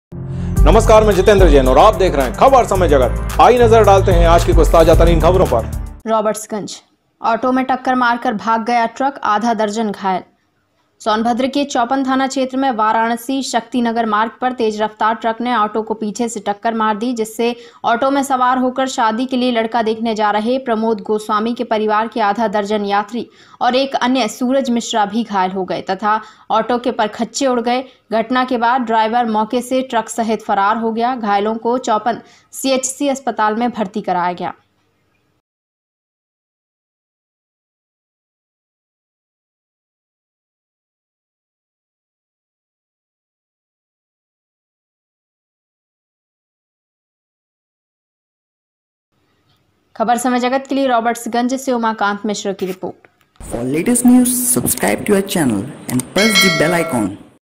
राबर्ट्सगंज- जा रहे थे लड़का देखने, पहुंच गए अस्पताल|| ऑटो में टक्कर मारकर भाग गया ट्रक,
उमाकांत मिश्र की रिपोर्ट